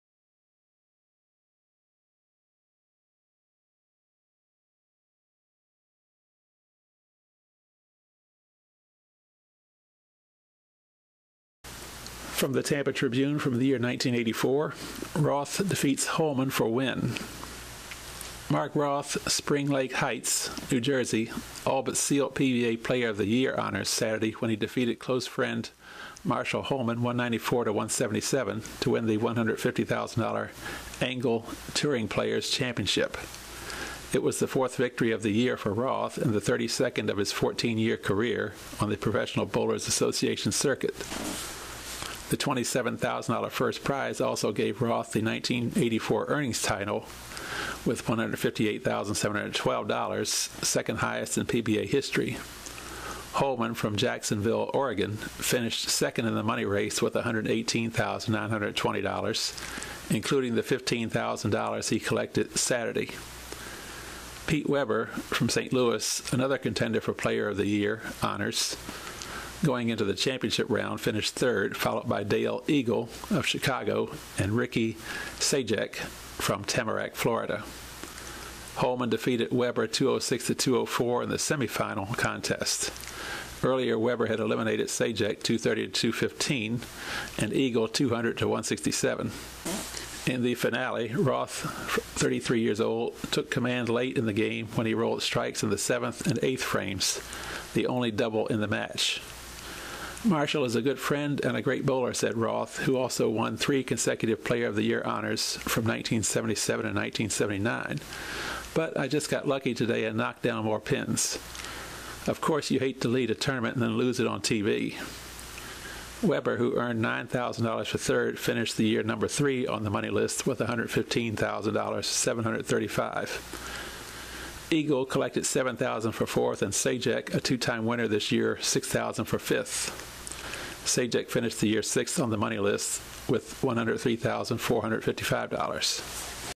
SpareTalk: Ep#20 - Roth Defeats Holman for Win (12/02/1984)
Rod Weaver, Author of the book on bowling history, "More Than Just a Game", discusses this day in bowling history (Episode 20).

Mark Roth defeats Marshall Holman for PBA victory.